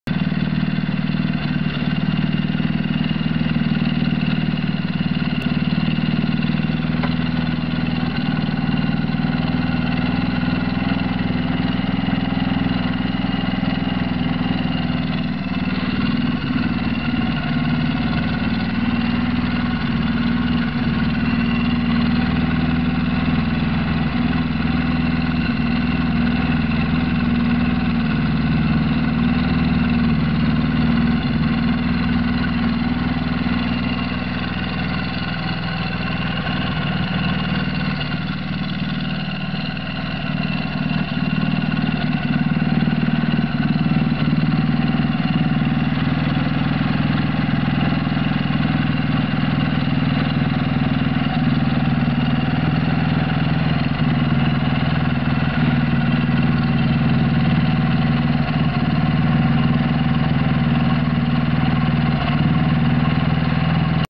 Citroën M35 nr. 122 Blue Delta engine
Overview of the running wankel comotor Citroën NSU engine.